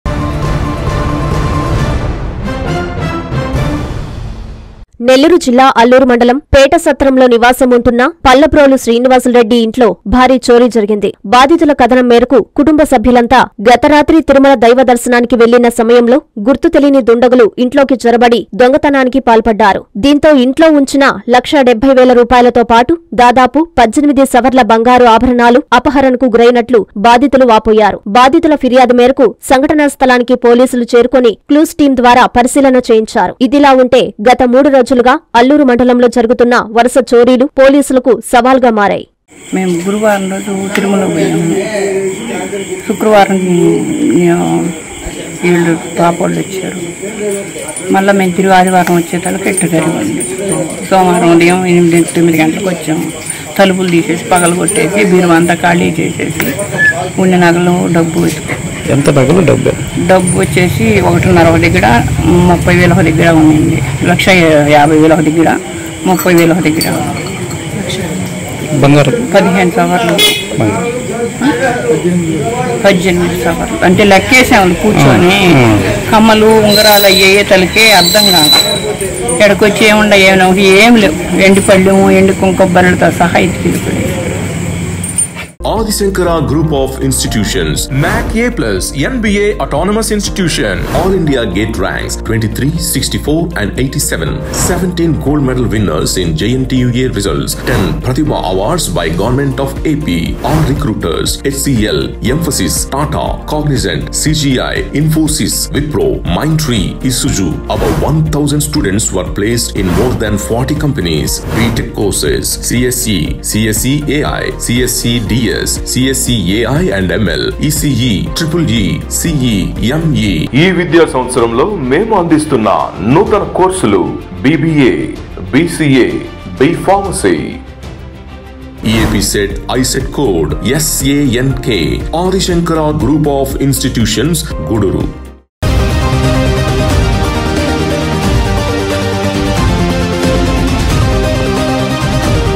అల్లూరులో భారీ చోరీ
- పేట‌స‌త్రంలోని ప‌ల్ల‌బ్రోలు శ్రీ‌నివాసులురెడ్డి ఇంట్లో రూ.1.70 ల‌క్ష‌ల న‌గ‌దు, 18 సవ‌ర్ల బంగారం అప‌హ‌ర‌ణ‌
- సంఘ‌ట‌న స్థ‌లాన్ని ప‌రిశీలించి కేసు న‌మోదు చేసి ద‌ర్యాప్తు చేస్తున్న పోలీసులు